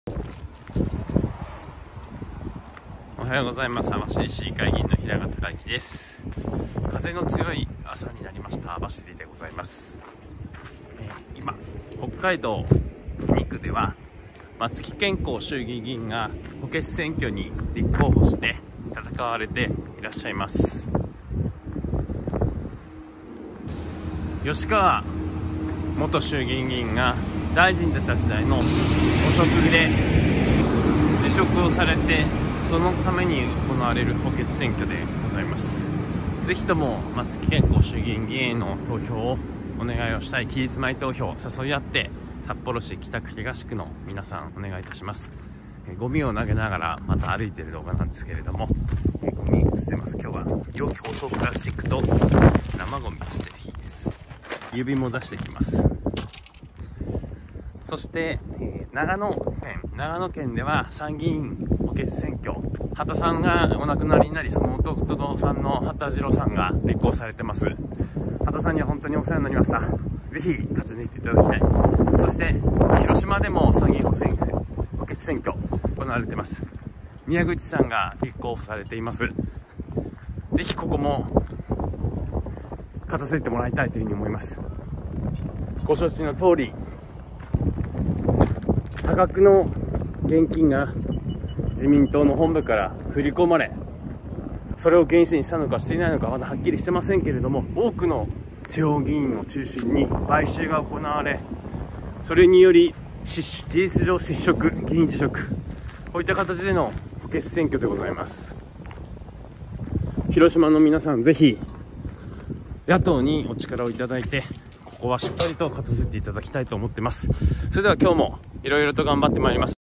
ゴミを投げる(捨てる)動画で3つの補欠選挙について話す
ゴミを投げる動画で、3つの補欠選挙について話しました。

吉川元農水大臣が現職大臣時代に汚職を行い辞職したことで行われる補欠選挙。

私も大変お世話になった 羽田雄一郎 参議が新型コロナ感染症により急逝されたため行われる補欠選挙。

河井元法相夫妻が多数の地方議員に対して買収を行い、裁判の経過でやっと議員辞職するなど多くの問題のある選挙。

自民党から1.５億もの政治資金が流れ、それが原資になったのか否か今もはっきりしない。